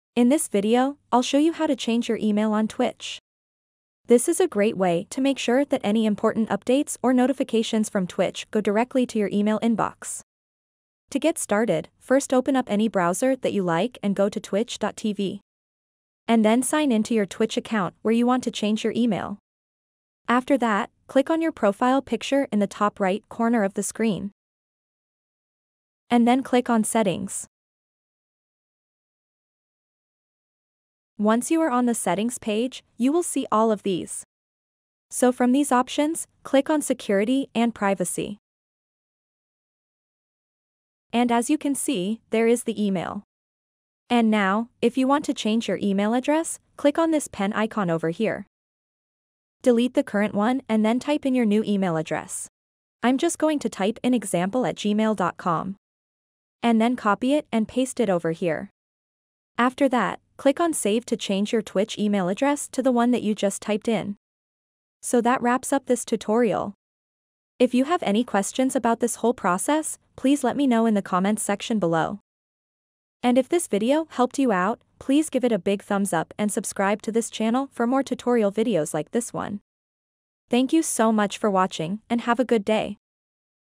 How To Change Email On Twitch
In this video, we walk you through the process of changing your email on Twitch. So sit back, relax, and follow along as we take you through the steps necessary to make this change!